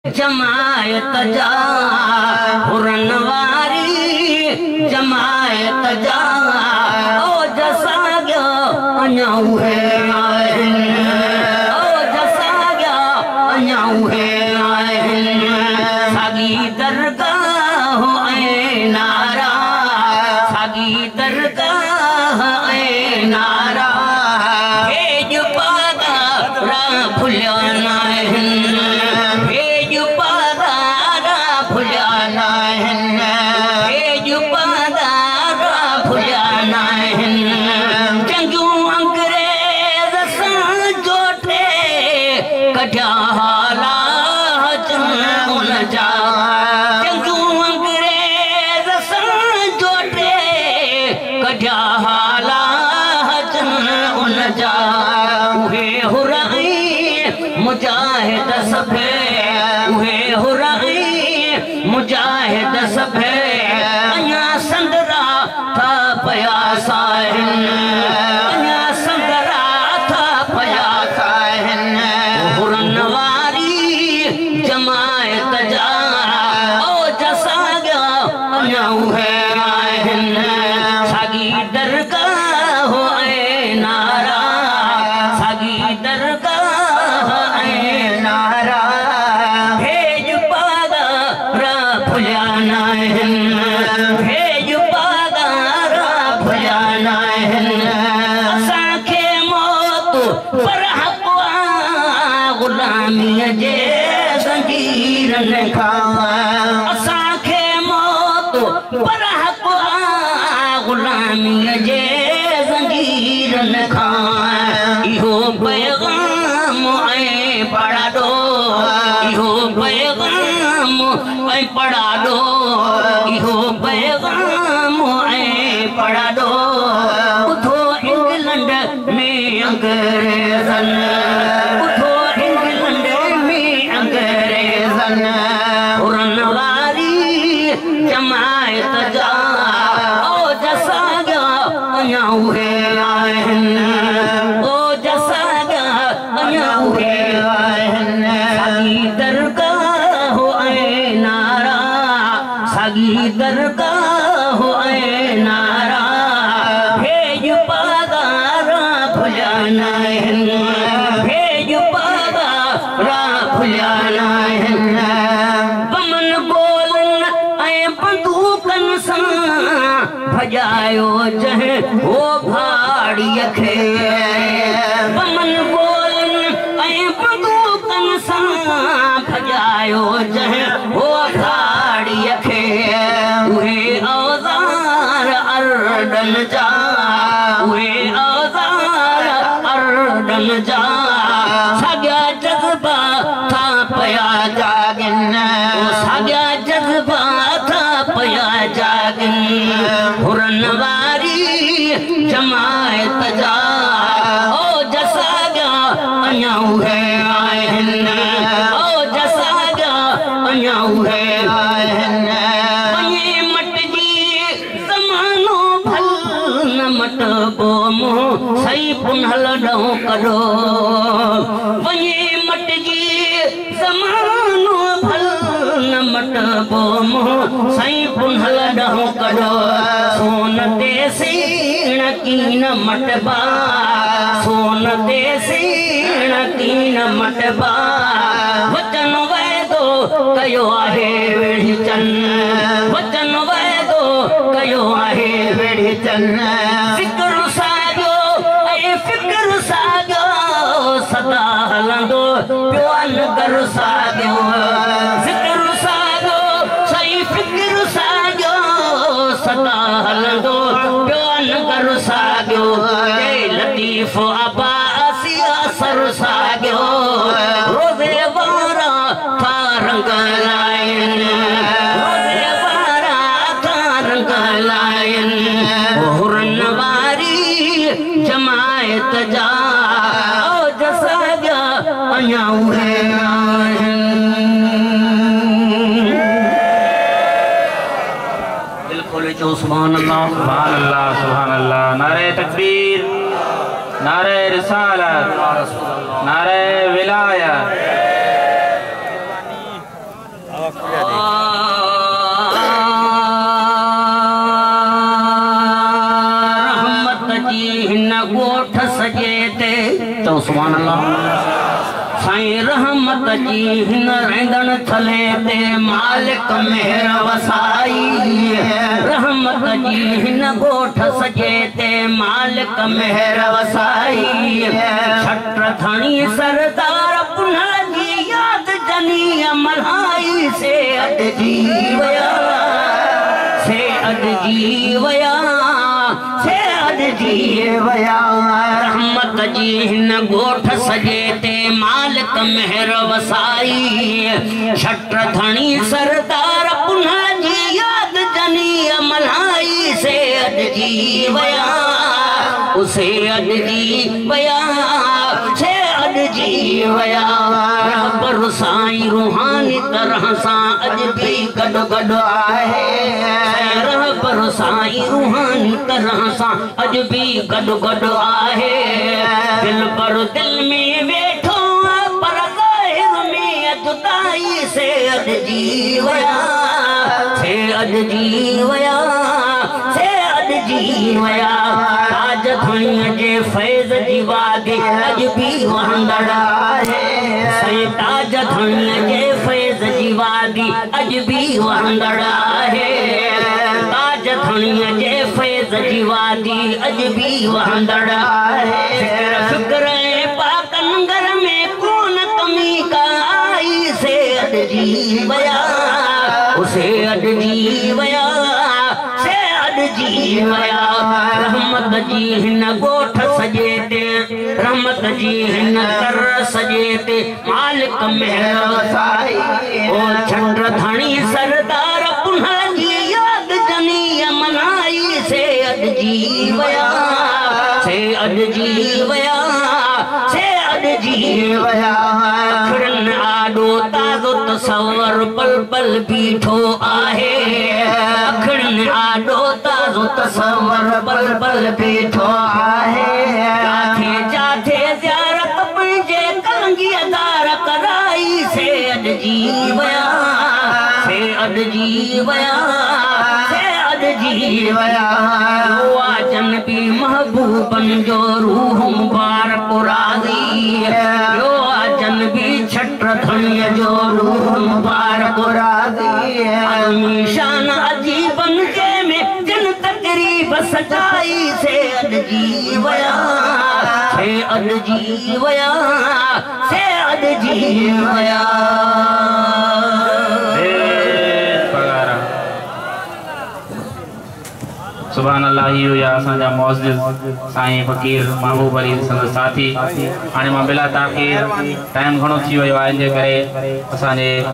Faqeer Mehboob Ali Shaikh Sindhi New Manqabat HD Molood By Hur Murtaza Baloch
Jashn e Eid Milad un Nabi Urs Mubarak
Hazrat Peer Syed Shah Mardan Shah
Chatt Dhani R.A
Huran Wari Jamait Ja
Faqeer Mehboob Ali Shaikh Sindhi New Naat HD
جشن عید میلاد النبی ﷺ
عرس مبارڪ حضرت پير سيد شاه مردان شاه ڇٽ ڌڻي
بتاريخ 23 آڪٽومبر بروز جمح جي شام ٿي گذريو
وڏو تعداد ۾ حر جماعت ۽ تمام عاشق رسول جي دوستن شرڪت ڪئي
 حرن واري جماعت جا اوج
 فقير محبوب علي شيخ